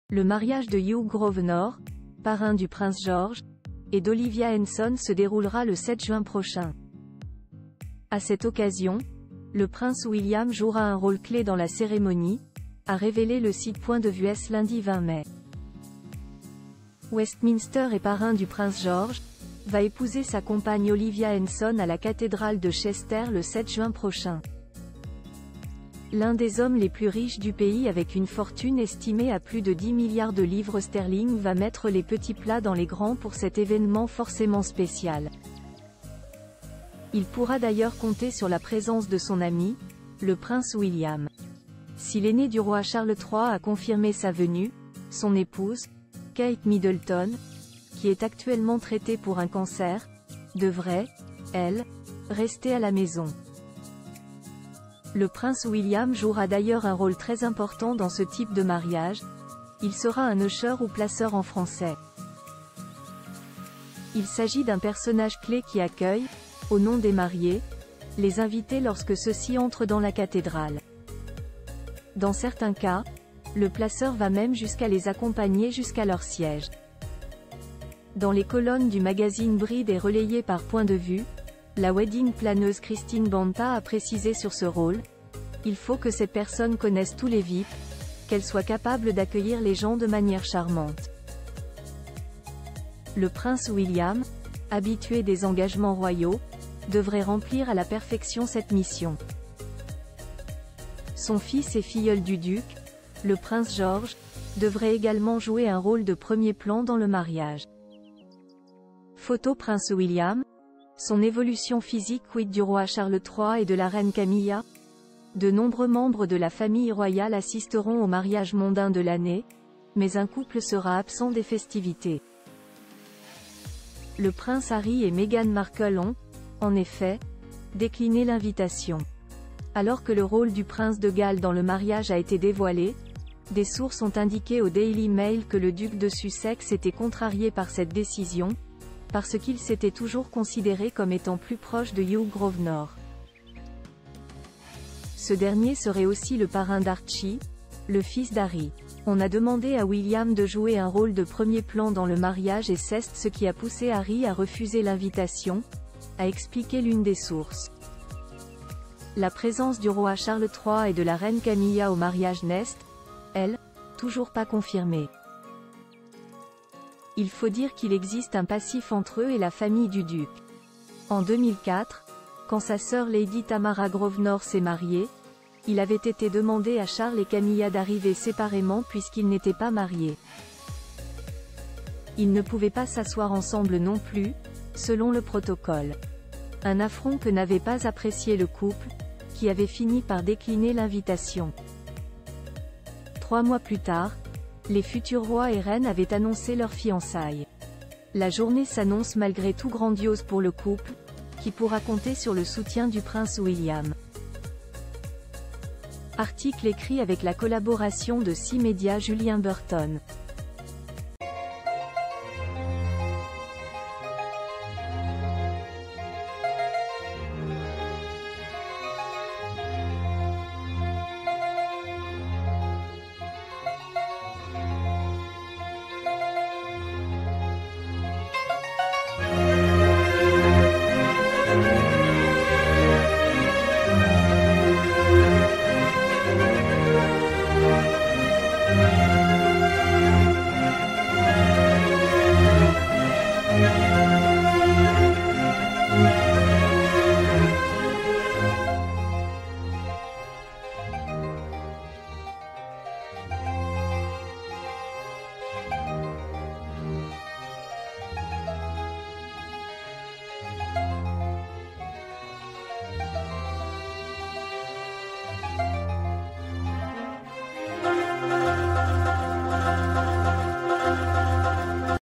Le prince William : son rôle clé au mariage de Hugh Grosvenor et Olivia Henson
Le mariage de Hugh Grosvenor, parrain du prince George, et d’Olivia Henson se déroulera le 7 juin prochain. À cette occasion, le prince William jouera un rôle clé dans la cérémonie, a révélé le site Point de vuece lundi 20 mai.    Westminster et parrain du prince George, va épouser sa compagne Olivia Henson à la cathédrale de Chester le 7 juin prochain. L’un des hommes les plus riches du pays avec une fortune estimée à plus de 10 milliards de livres sterling va mettre les petits plats dans les ...